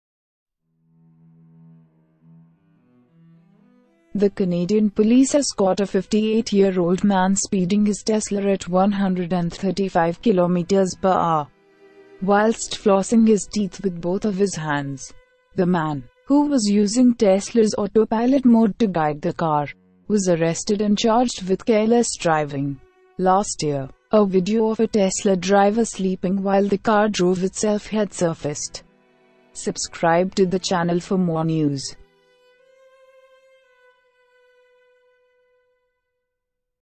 Tesla driver caught using Autopilot at 135 kmph to floss teeth, arrested
The Canadian police has caught a 58-year-old man speeding his Tesla at 135 kilometres per hour whilst flossing his teeth with both of his hands. The man, who was using Tesla's Autopilot mode to guide the car, was arrested and charged with careless driving. Last year, a video of a Tesla driver sleeping while the car drove itself had surfaced.